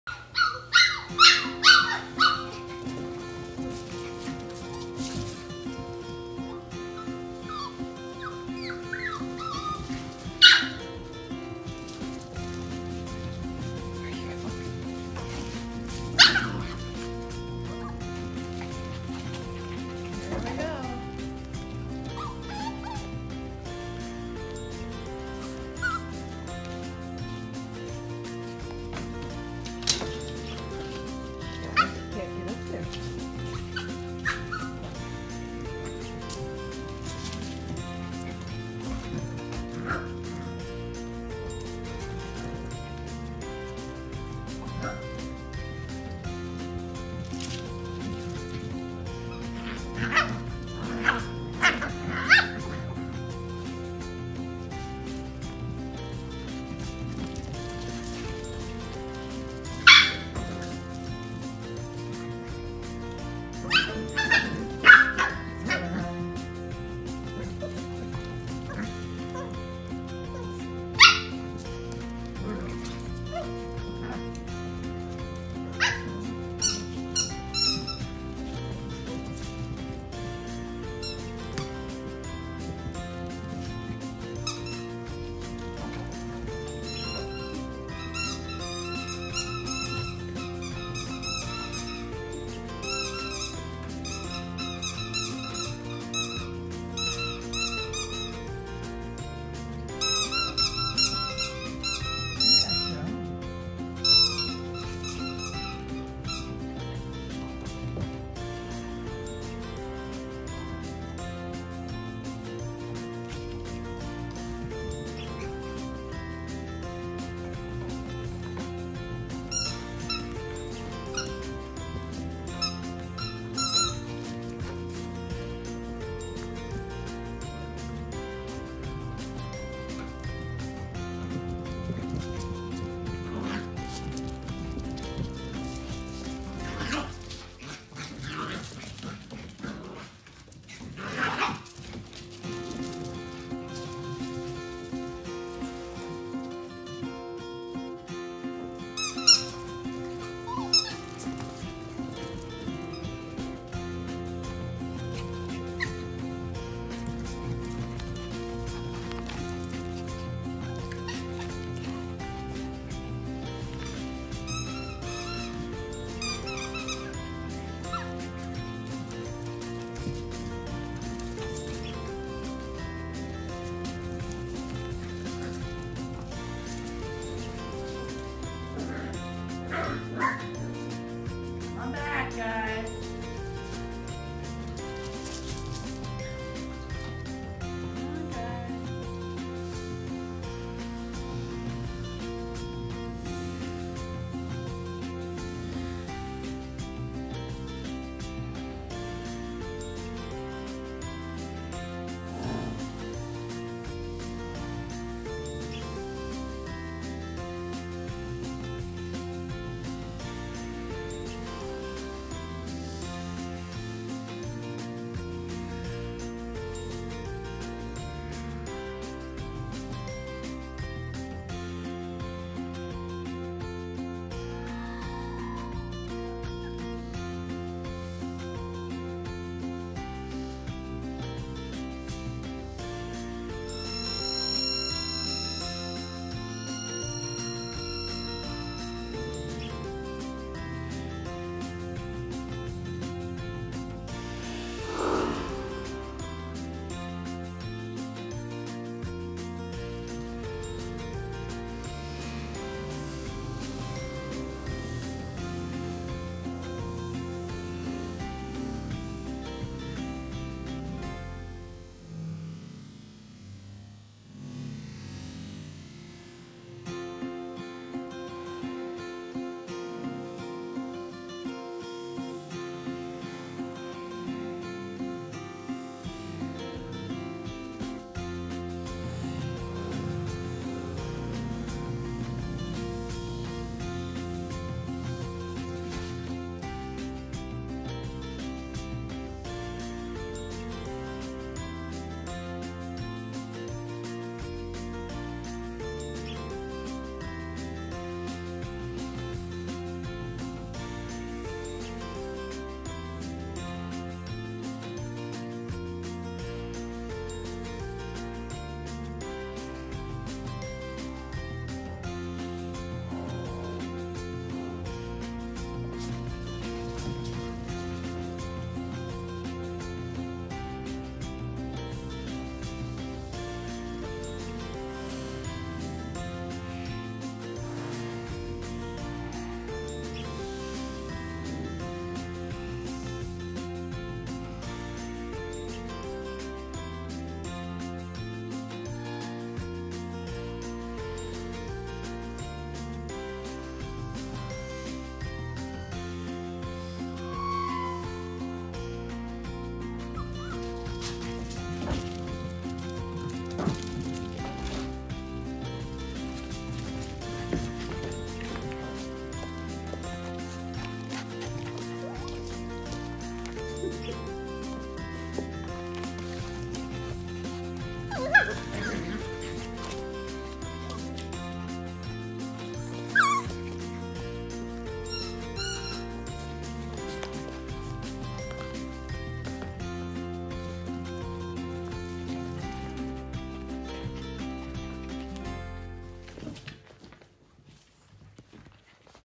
Micro mini sheepadoodle 8 weeks old
Micro mini sheepadoodle, 8 weeks old, they have a wavy hypoallergenic coat, should mature 12-22 pounds,  great size  for travel and indoor living.  Athletic, smart,  great little companions!